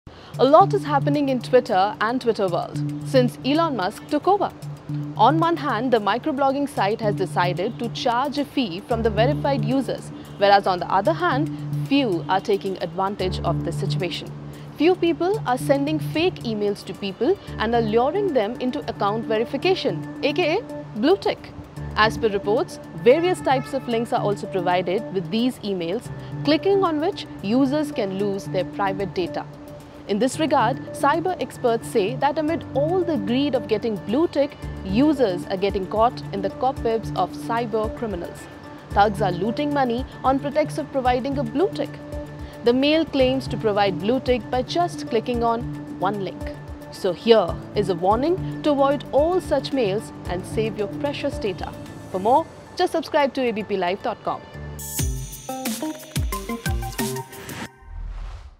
Blue Tick: Beware of these e-mails offering verified Twitter accounts | ABP LIVE
A lot is happening in the Twitter world since Elon Musk took over. On one hand, the microblogging site has decided to charge a fee from the verified users whereas on the other hand few are taking advantage of this situation. Few people are sending fake emails to people and are luring them into account verification aka blue tick.
As per reports, various types of links are also provided with these emails, clicking on which users can lose their private data.

In this regard, cyber experts say that amid all the greed of getting blue ticks, users are getting caught in the cobwebs of cyber criminals. Thugs are looting money on pretext of providing a blue tick. The mail claims to provide blue tick by just clicking on a link.

So here is a warning to avoid all such mails and save your precious data. For more